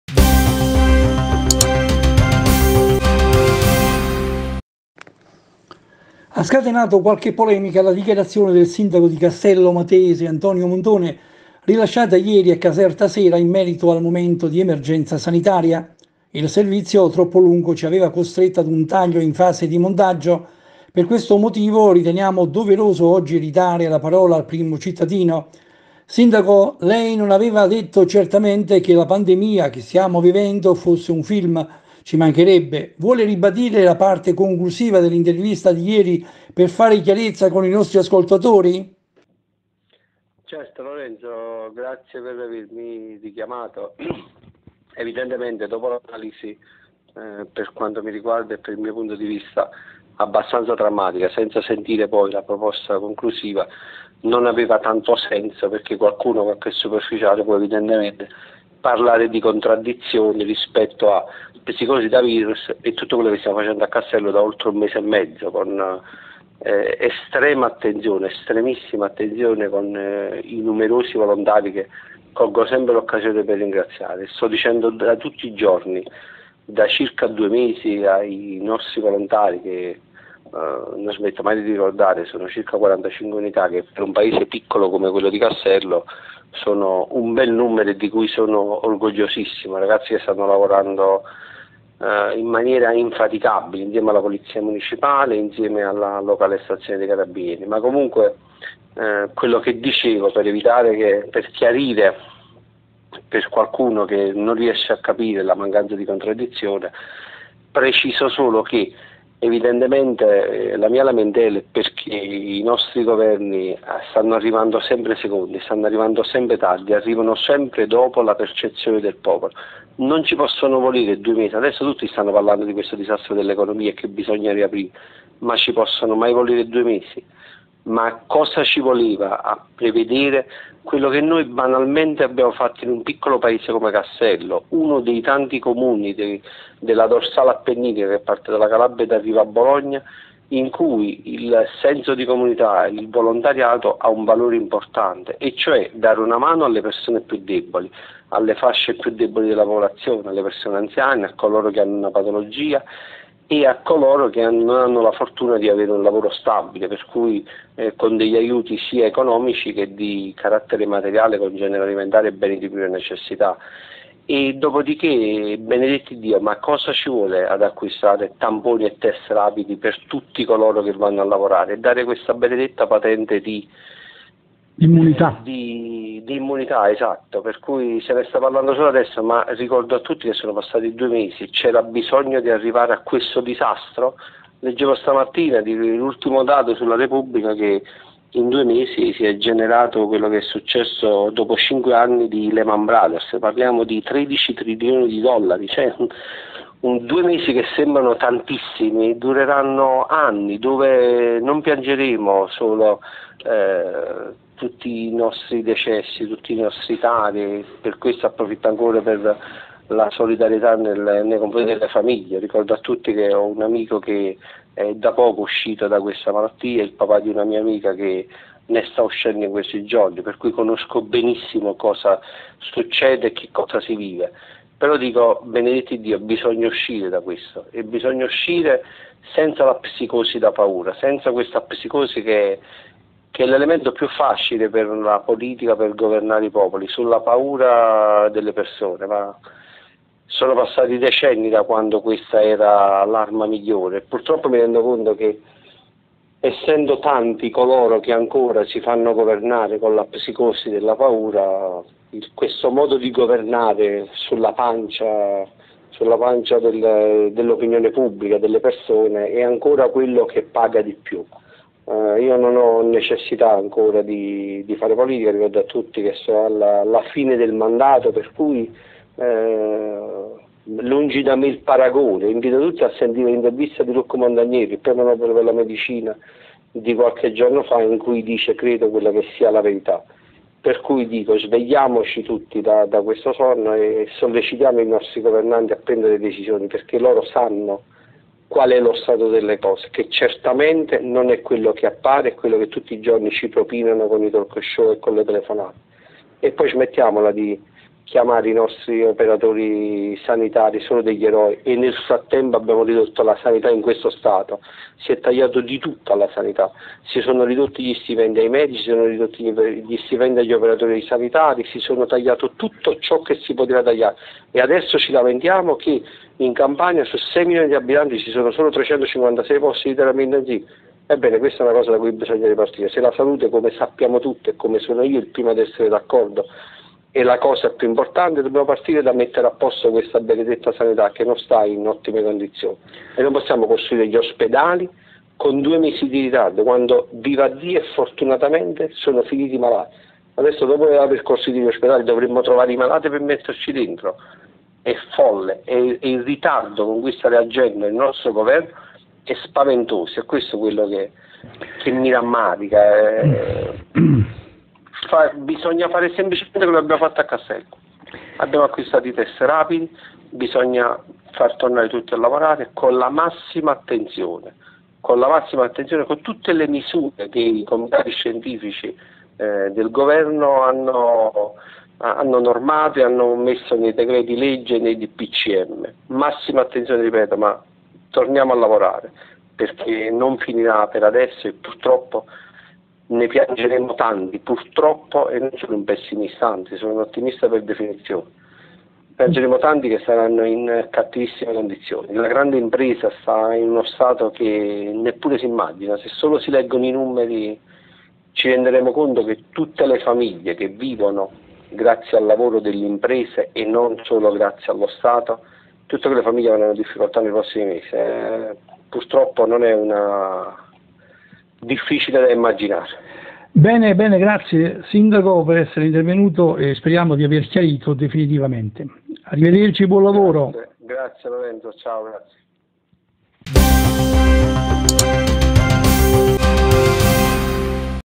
Casertasera.it Castello Matese, il sindaco precisa.Evitare la psicosi e rispettare le regole
Il sindaco di Castello del Matese, precisa cio' che aveva dichiarato ieri a casertasera.it e ribadisce il pieno rispetto del decreto anti covid  ma evitare la psicosi è fondamentale.